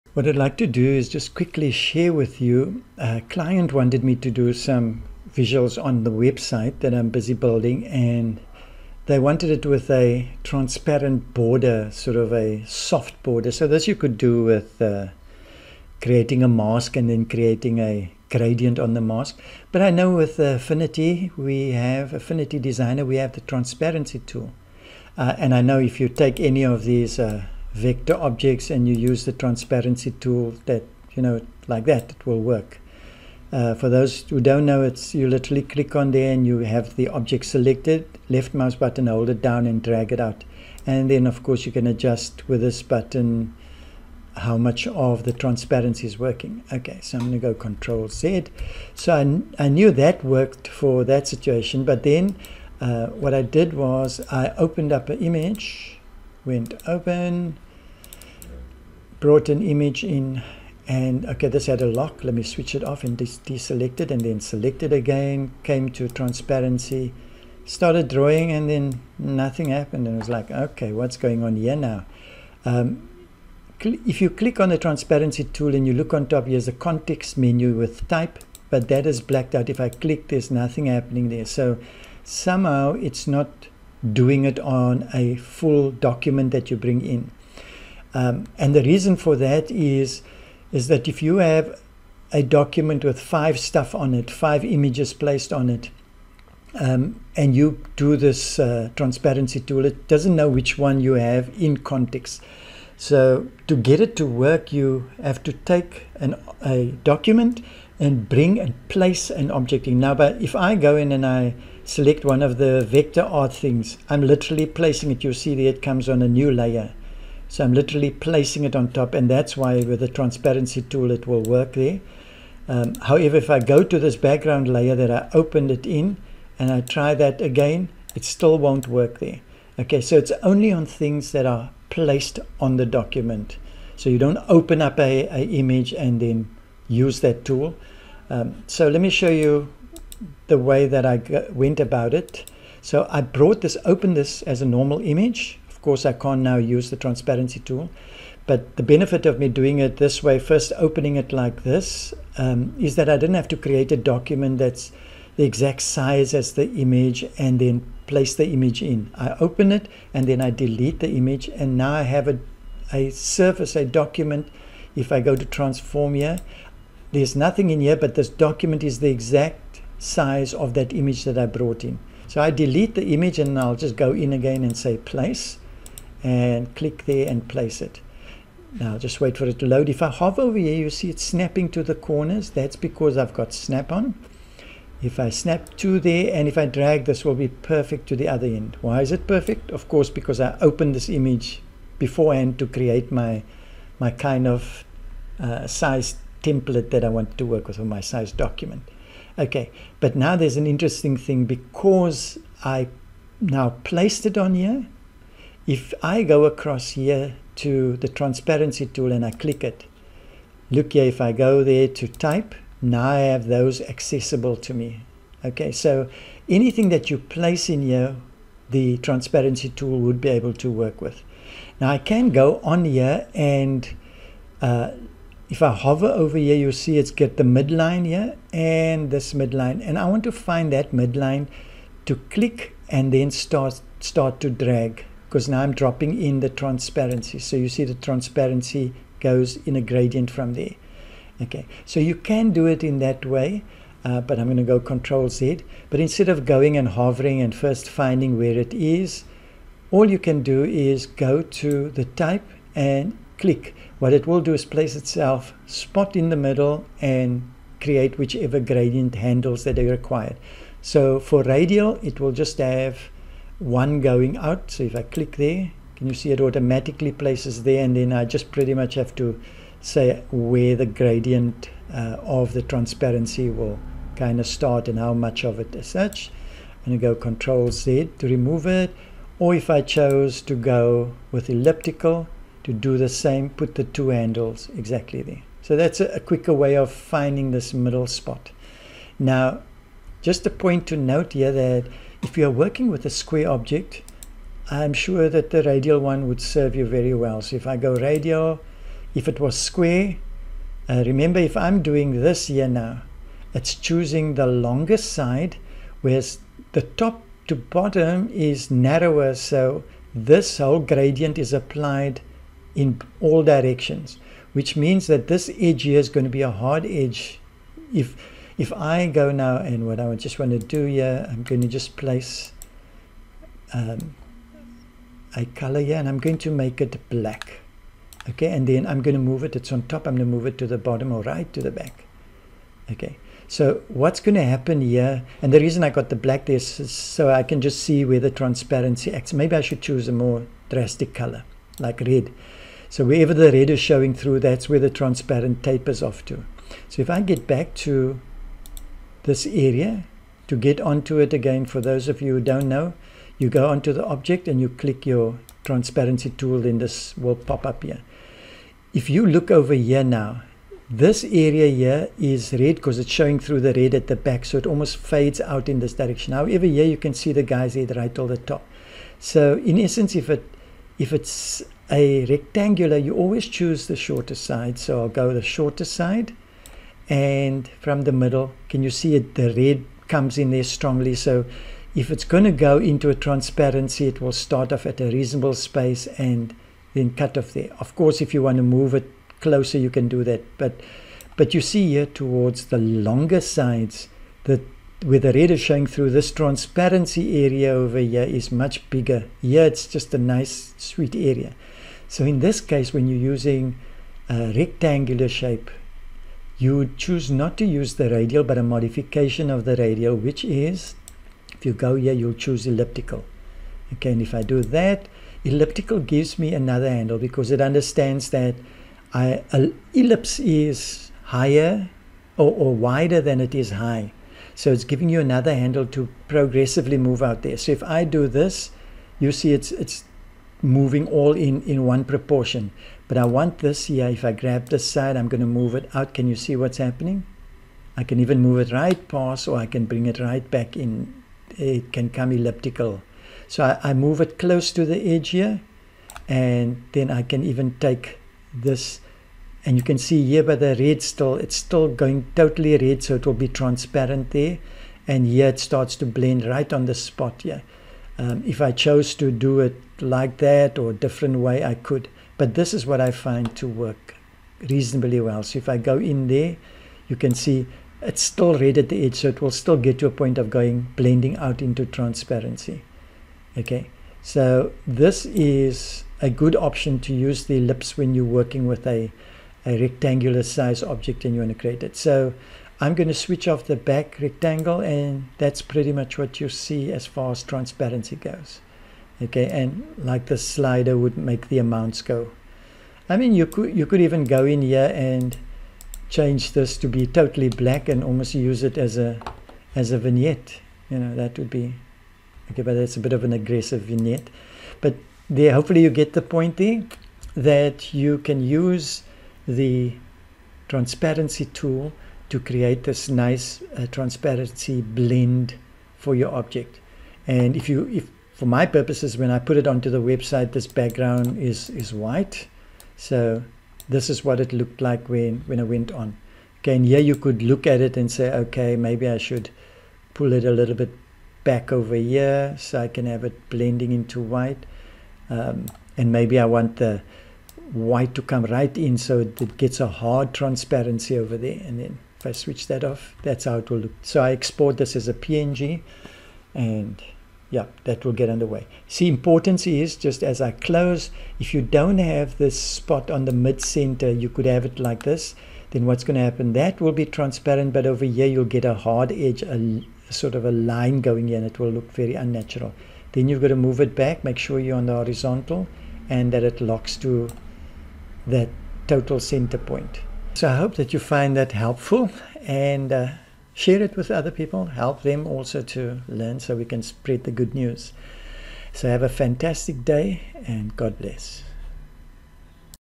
Transparency in Affinity Designer
Creating Transparency gradient on Pixel Based Images without a Mask in Affinity Designer.  Its a really great tool to have when you wanting to master Gradient Transparent on Vector or Pixel Based Objects ( Image)